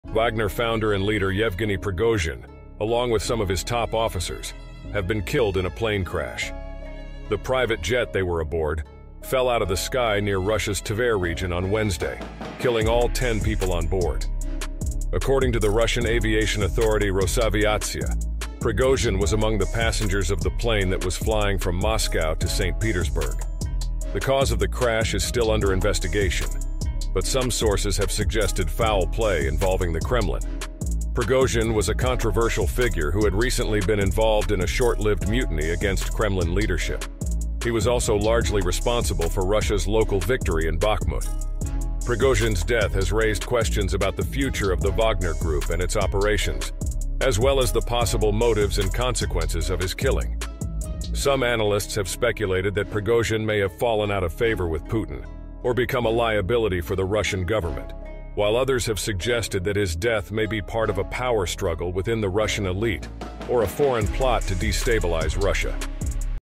Wagner leader Yevgeny Prigozhin dead after plane crash.
Wagner founder and leader Yevgeny Prigozhin, along with some of his top officers, have been killed in a plane crash. The private jet they were aboard, fell out of the sky near Russia’s Tver region on Wednesday, killing all 10 people on board. According to the Russian aviation authority Rosaviatsiya, Prigozhin was among the passengers of the plane that was flying from Moscow to St. Petersburg. The cause of the crash is still under investigation, but some sources have suggested foul play involving the Kremlin. Prigozhin was a controversial figure who had recently been involved in a short-lived mutiny against Kremlin leadership. He was also largely responsible for Russia’s local victory in Bakhmut. Prigozhin’s death has raised questions about the future of the Wagner Group and its operations, as well as the possible motives and consequences of his killing. Some analysts have speculated that Prigozhin may have fallen out of favor with Putin or become a liability for the Russian government, while others have suggested that his death may be part of a power struggle within the Russian elite or a foreign plot to destabilize Russia. What do you think will happen next?